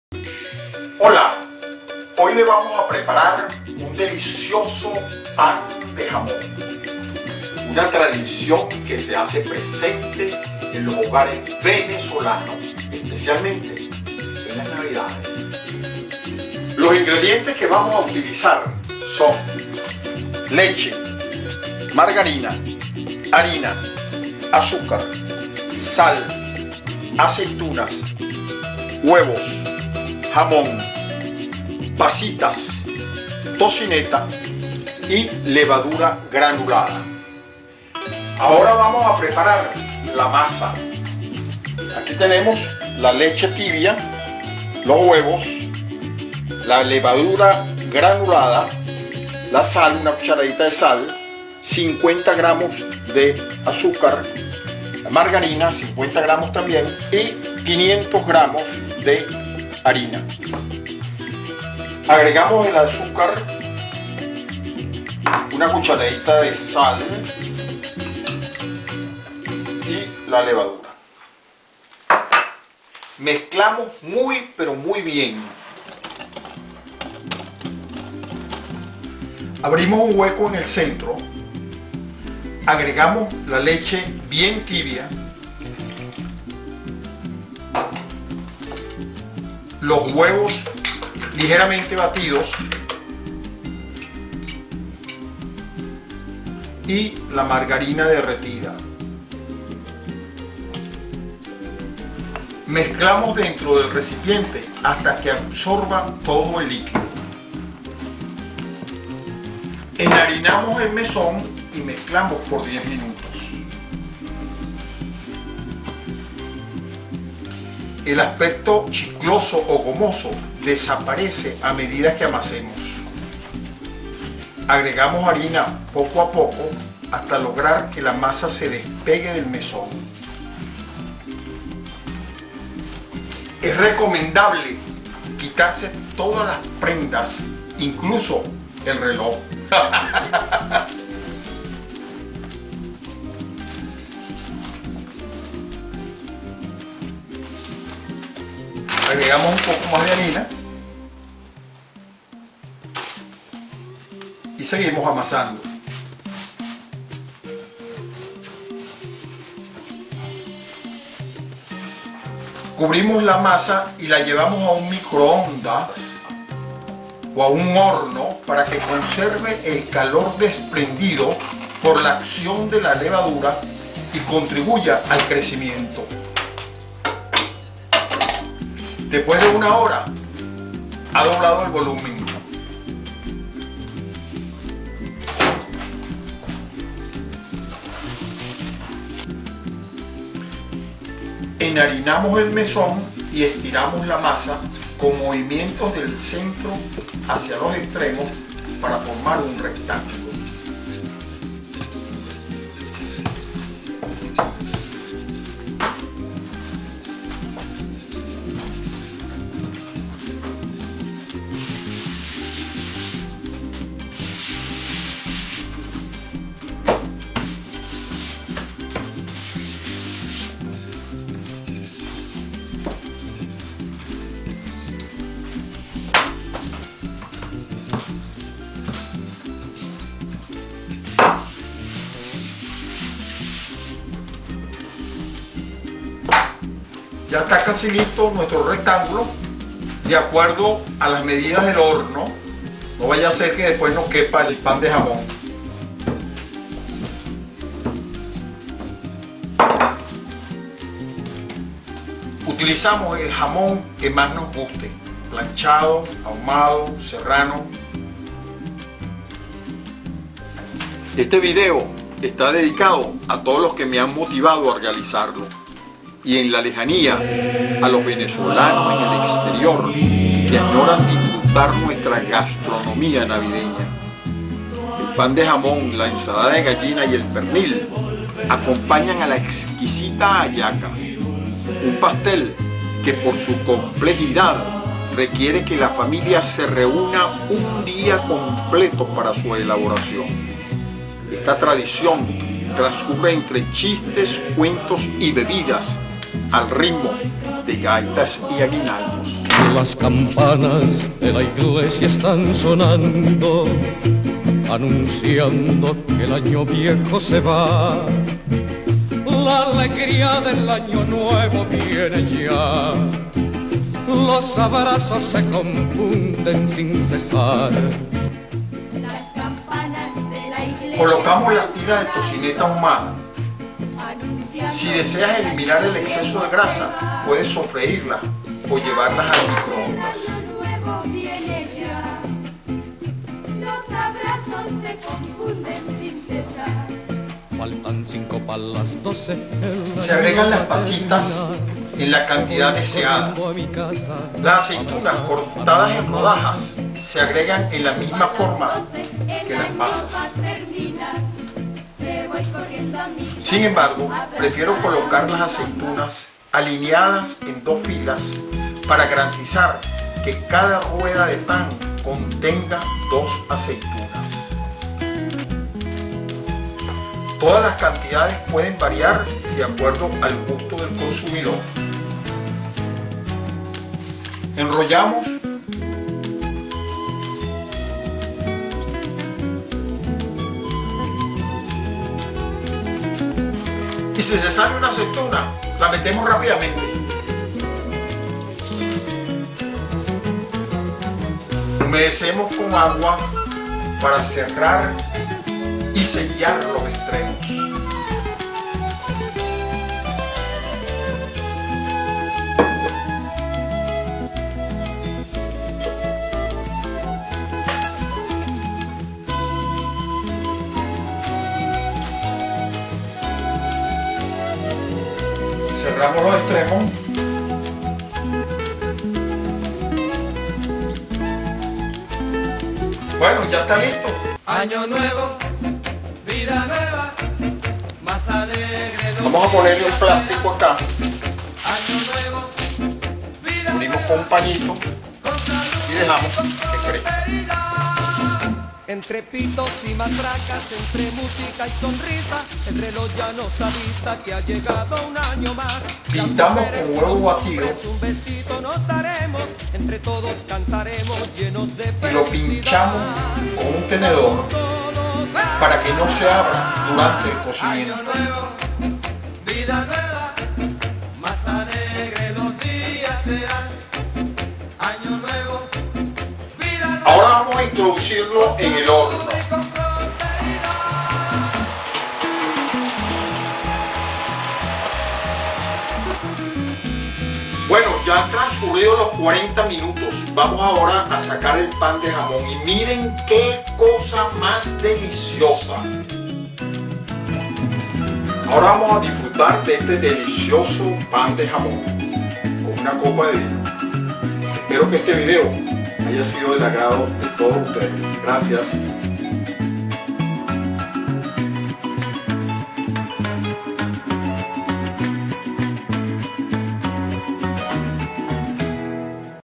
Ham Bread - Pan de Jamón Maximiliano.
This is a delicious venezuelan traditional Christmas ham bread. The dough is lightly sweetened and filled with ham, streaky smoked bacon, raisins and olives. May also contains strips of red or green peppers and a few capers.